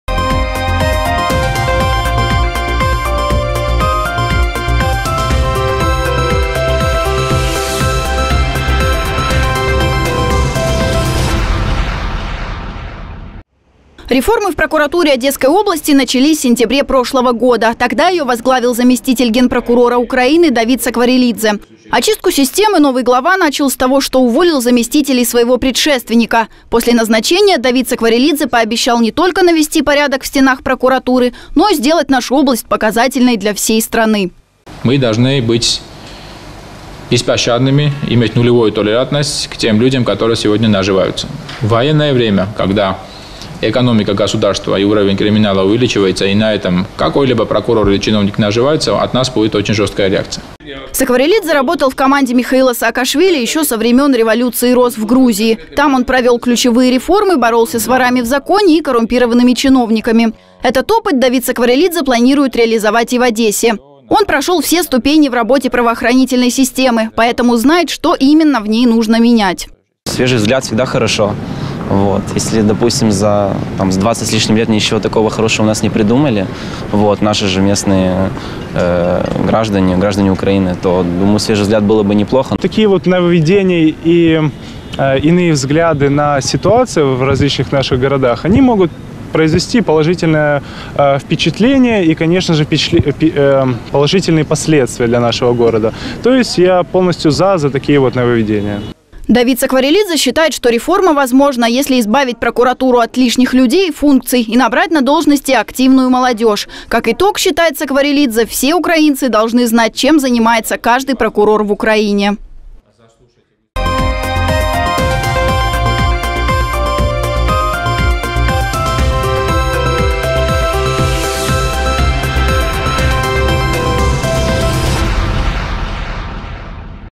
Репортаж: реформы в прокуратуре
Реформы в прокуратуре Одесской области начались в сентябре прошлого года. Тогда ее возглавил заместитель генпрокурора Украины Давид Сакварелидзе. Очистку системы новый глава начал с того, что уволил заместителей своего предшественника. После назначения Давид Сакварелидзе пообещал не только навести порядок в стенах прокуратуры, но и сделать нашу область показательной для всей страны.